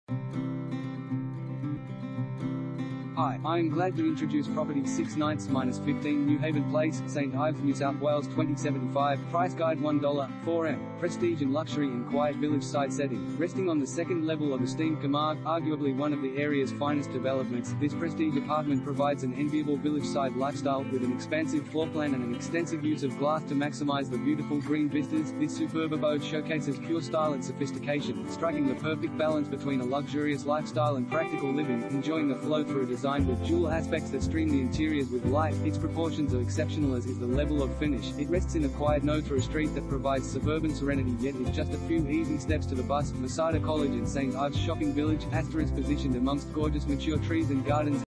6/9-15 Newhaven Place, St Ives NSW 2075 | AuPropertyReport.Com
6/9-15 Newhaven Place, St Ives NSW 2075
Property Type: apartment
Bedrooms: 3
Bathrooms: 3
Car Space: 2
Price Guide $1.4M - Prestige and luxury in quiet village-side setting!